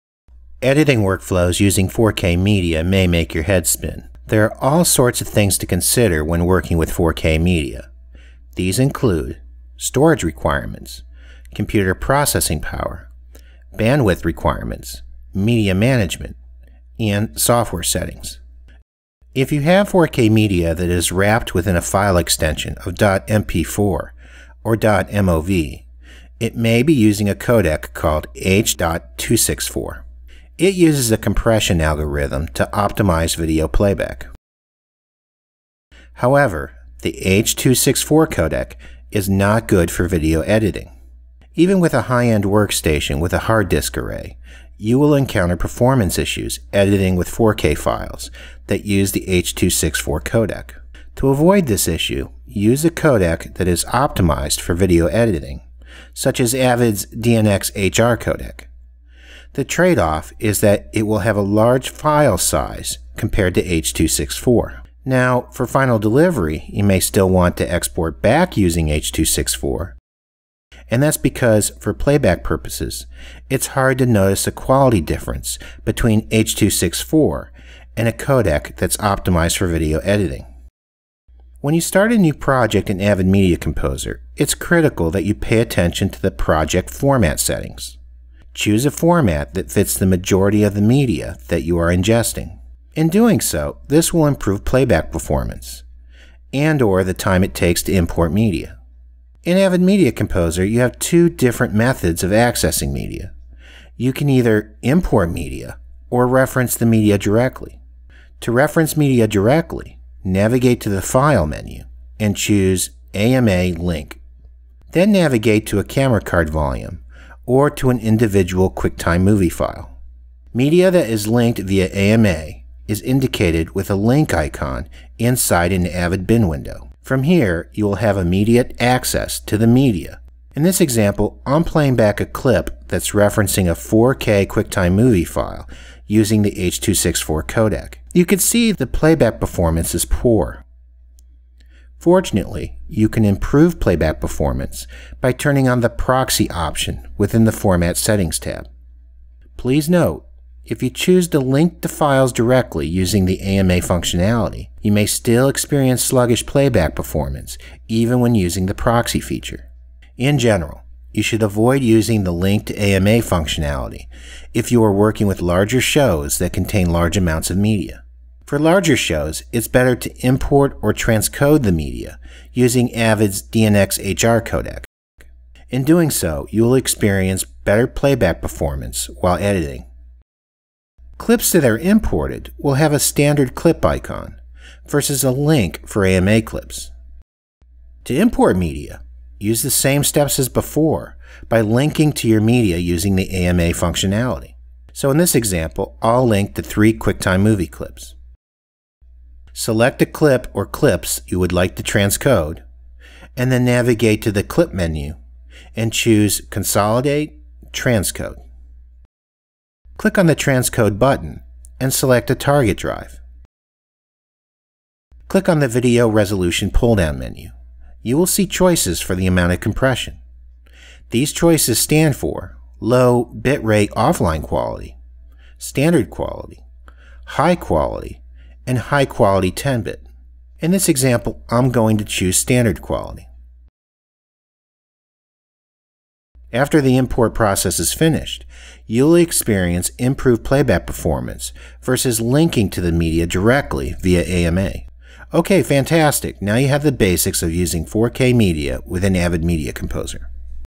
Avid Media Composer 4K workflow
Learn how to link or import 4K media into Avid Media Composer.  Learn why it’s important to transcode media that uses the H.624 codec.